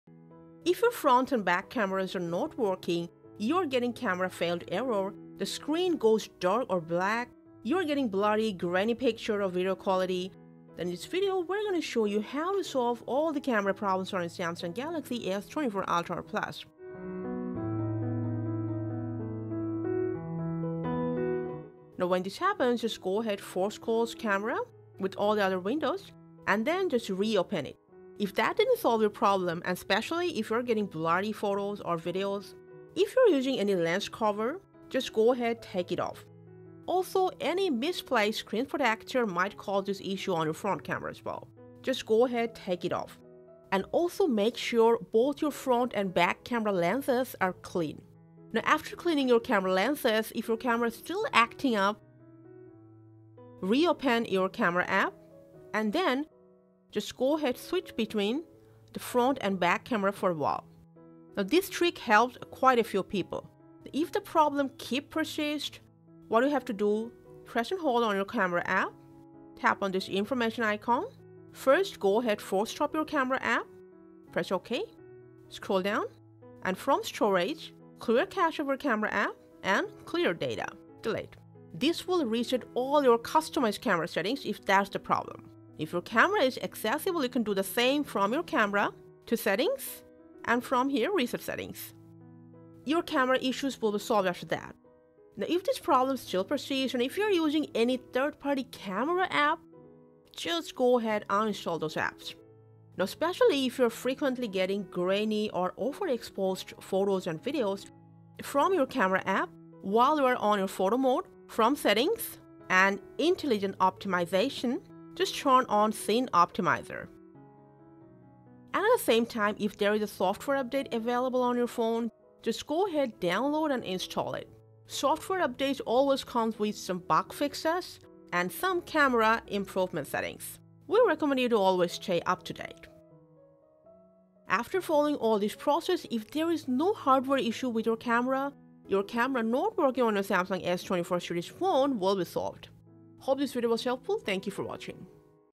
Fix- Camera Not Working on Samsung Galaxy S24 Ultra! [Blurry]
Samsung Galaxy S24/S24+/S24 Ultra Camera taking Blurry Pictures? Struggling with various Camera issues on your Galaxy S24 series phones such as,

*Galaxy S24 Camera Black screen,
* Camera Failed error,
* Front Camera not working,
* Camera out of focus,
* Overexposed Photos,
* Camera app keeps crashing,
* Camera app not opening,
* Camera Zoomed in or out

Don’t panic, the Fix369 Team brought your 5 straightforward solutions to fix the Camera not working problem on the Samsung Galaxy S24 Ultra/Plus effectively.

0:00 Samsung Galaxy S24 Ultra/Plus Camera Issues
0:21 Solution 1: Basic Troubleshooting
0:53 Solution 2: Switch Cameras a few times
1:05 Solution 3: Clear Cache and Data/Reset Camera Settings
1:35 Solution 4:  Remove third-party camera apps
1:44 Solution 5: Change Camera Settings and Software update.